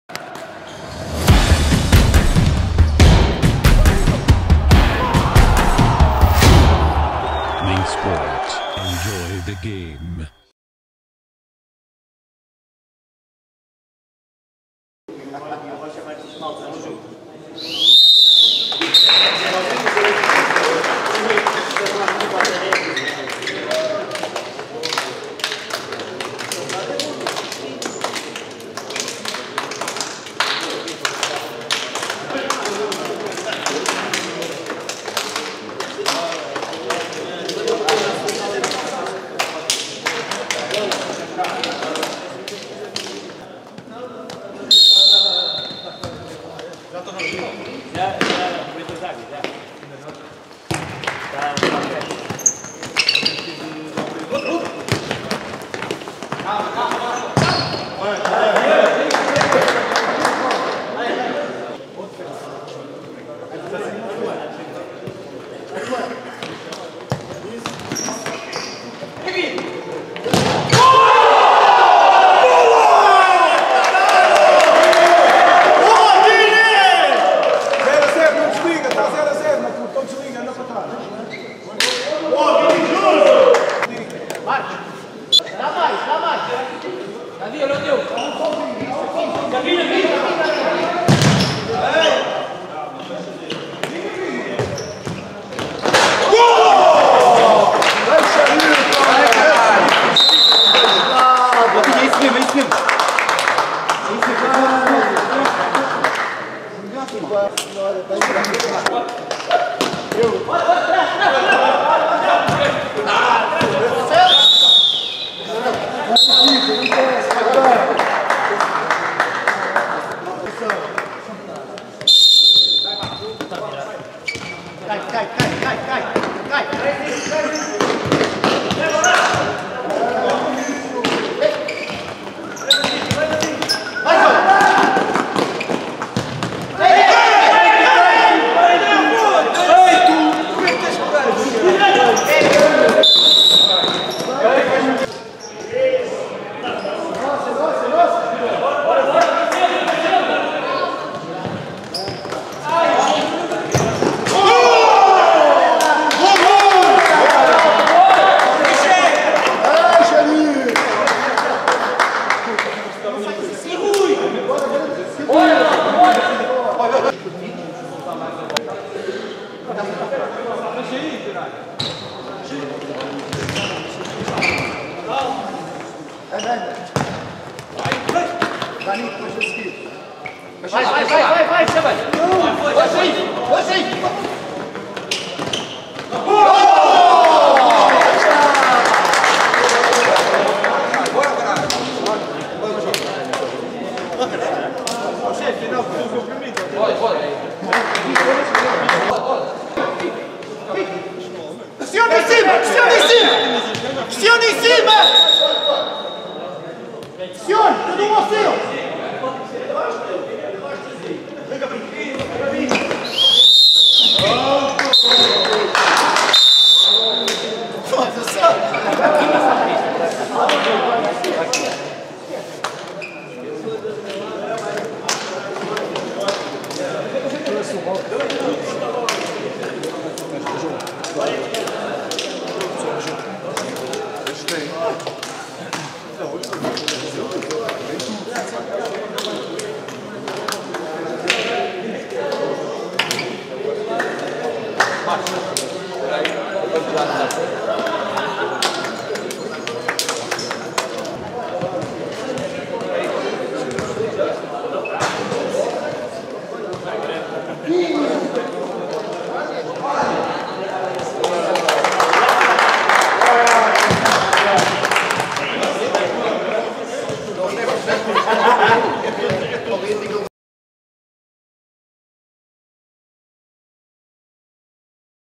RESUMO-GRCD LEIÃO vs NC FUTSAL 1-3 XXI Edição Liga Corporate Futsal Linksport 2ªDiv-B 9ªJorn
RESUMO
GRCD LEIÃO vs NC FUTSAL 1-3
 " XXI Edição Liga Corporate Futsal Linksportt" 2ªDiv-B 9ªJorn
" Escola Secundária Seomara da Costa Primo" Amadora
Grafismo: Fluctus "Carlos Ferreira"
Gravação e Edição: Linkvídeo "Rui Soares"
23/11/2023